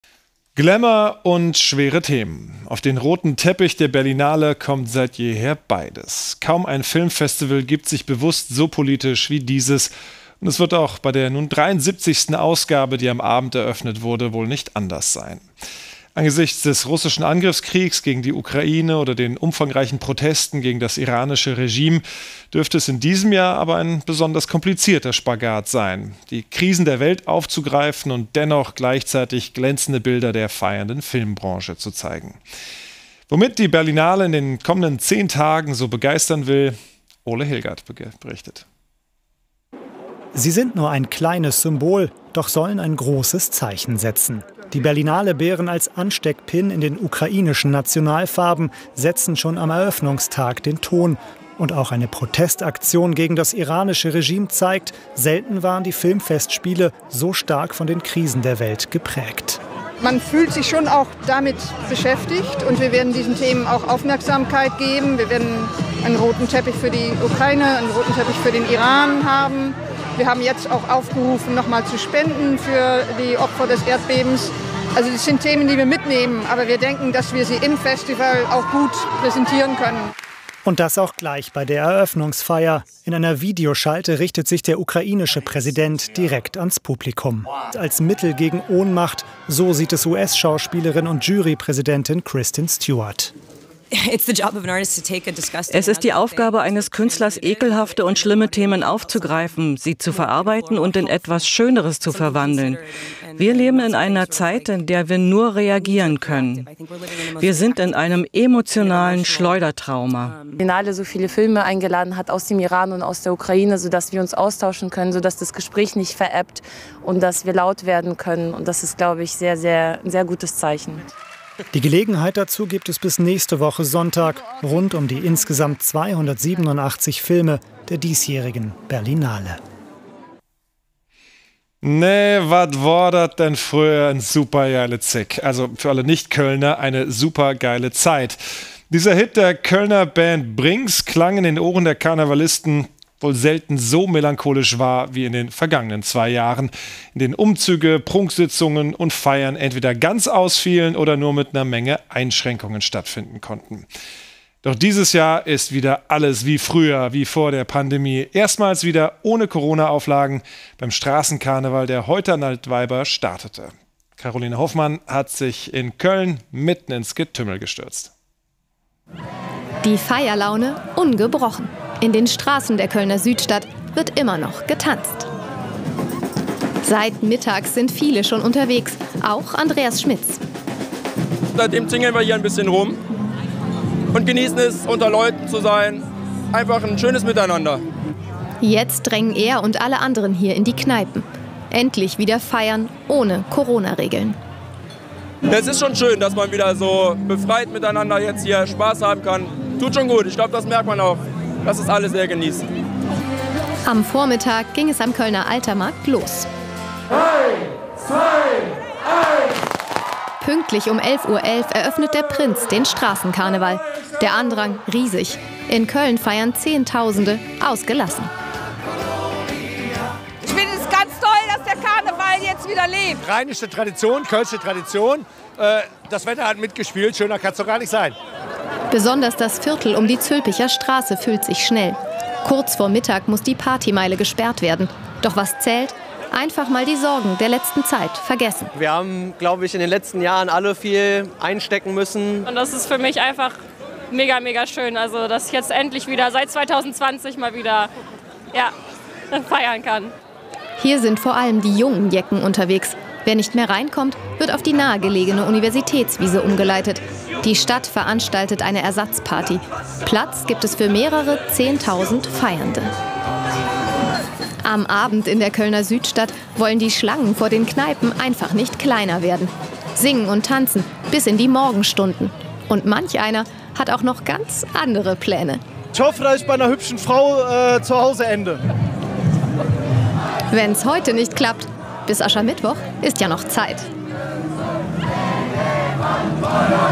Auftakt der 73. Berlinale 2023, Straßen-Karneval in Köln gestartet.
Auftakt der 73. Berlinale,
Straßen-Karneval in Köln gestartet.

Hinweis:
Der Beitrag zum Thema „Auftakt der 73. Berlinale“ darf aus rechtlichen Gründen nicht vollständig auf tagesschau.de gezeigt werden.